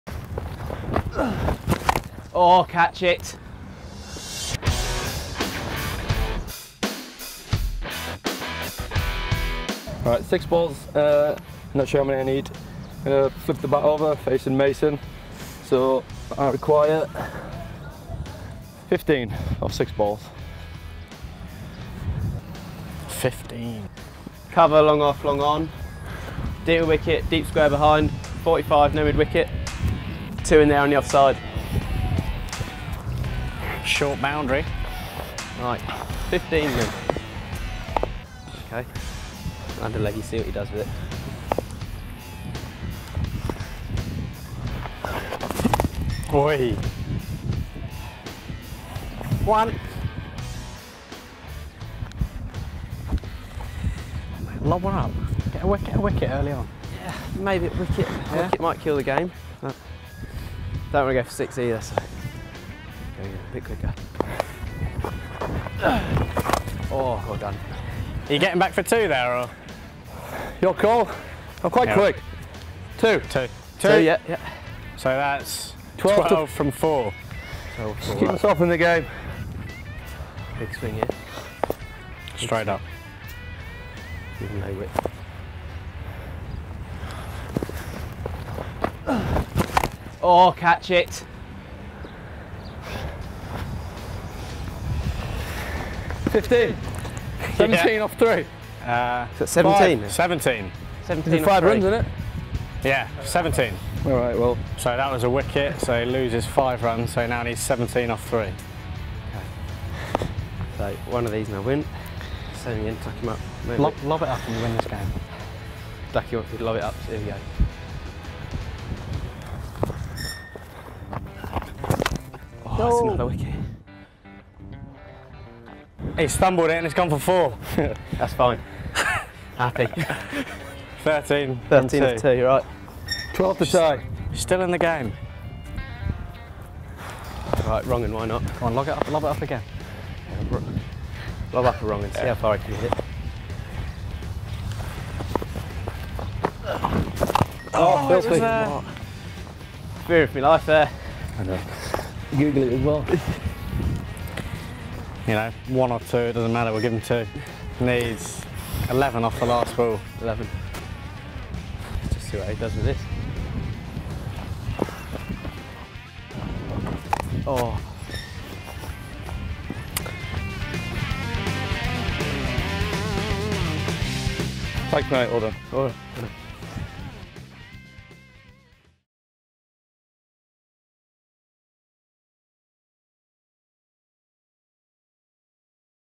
Liam Plunkett vs Mason Crane - 15 runs off 6 balls | Net Battles
Liam Plunkett steps up to the crease and faces an over from Mason Crane. Can the England cricketer score 15 runs off 6 balls?